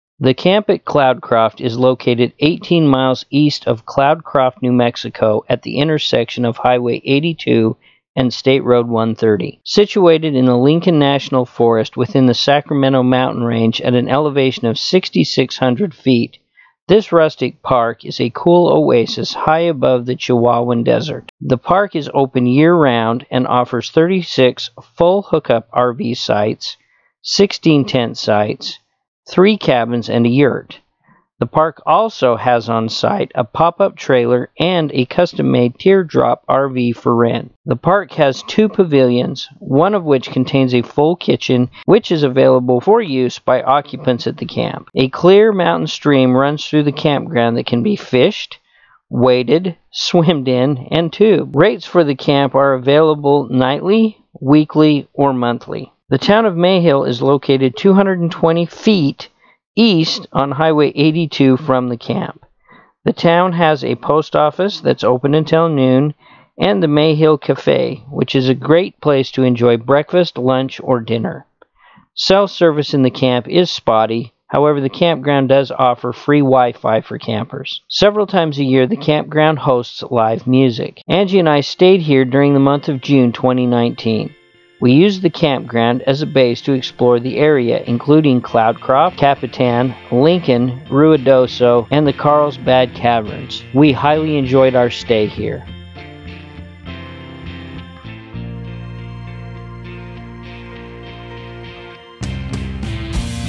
RV Park New Mexico Camp@Cloudcroft Review
RV Parks in New Mexico, The Camp @ Cloudcroft Campground review of Camp@Cloudcroft.

RV Park in New Mexico. We spent a month at this RV Park just outside of Mayhill, New Mexico in June 2019.

We explored southern New Mexico while staying at this RV Park, including Cloudcroft New Mexico, Ruidoso New Mexico, Lincoln City New Mexico, Alamagordo New Mexico.